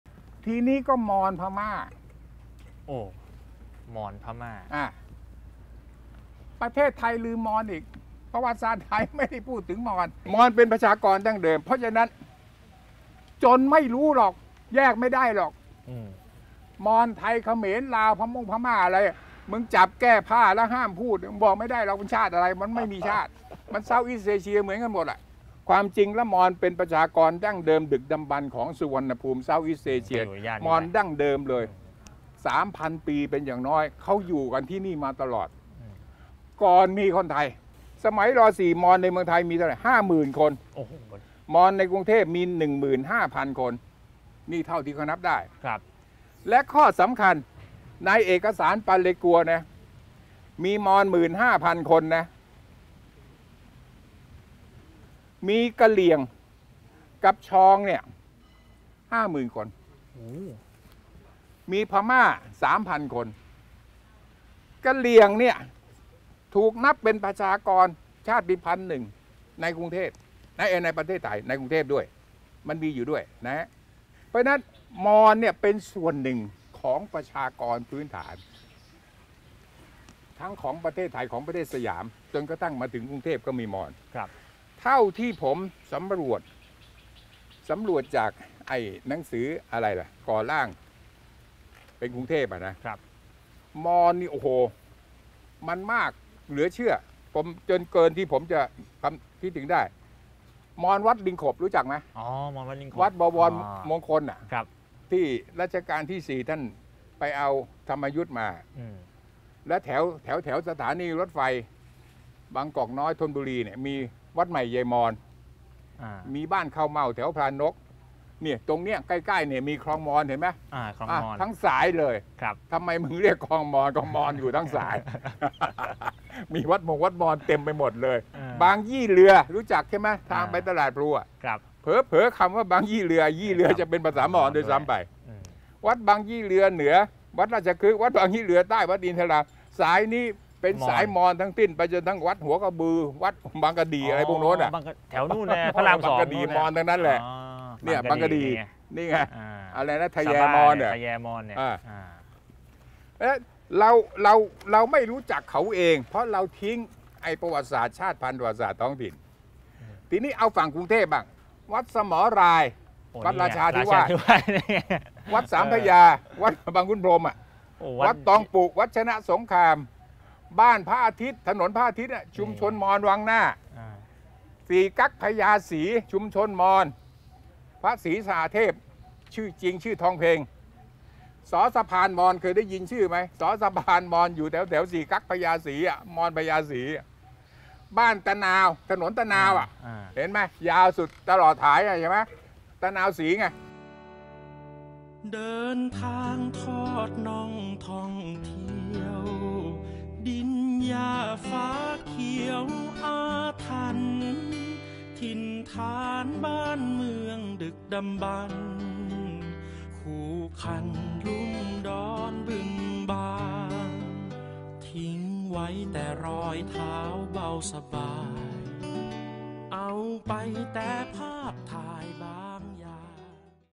ขรรค์ชัย-สุจิตต์ ทอดน่องท่องเที่ยว : คนมอญในกรุงเทพฯ อยู่ตรงไหน?
ย้อนประวัติศาสตร์ท้องถิ่นกรุงเทพฯ  คนมอญอยู่ตรงไหน?

เดินทางทอดน่องท่องเที่ยวเพื่อศึกษาเรียนรู้ประวัติศาสตร์-โบราณคดี-วัฒนธรรมของสังคมไทย ไปกับ 2 กุมารสยาม "ขรรค์ชัย บุนปาน - สุจิตต์ วงษ์เทศ"